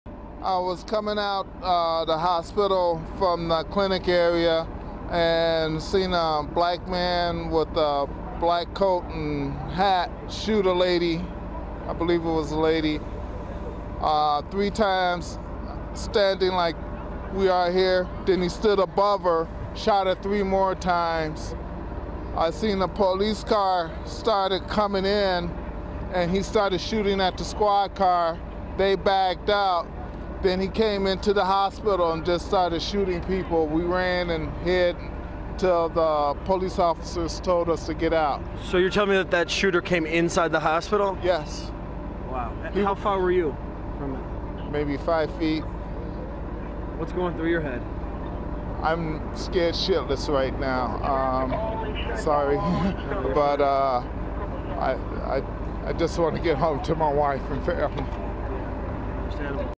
Mercy Hospital Shooting Witness
A man describes watching a man dressed in black shoot a woman at least three times.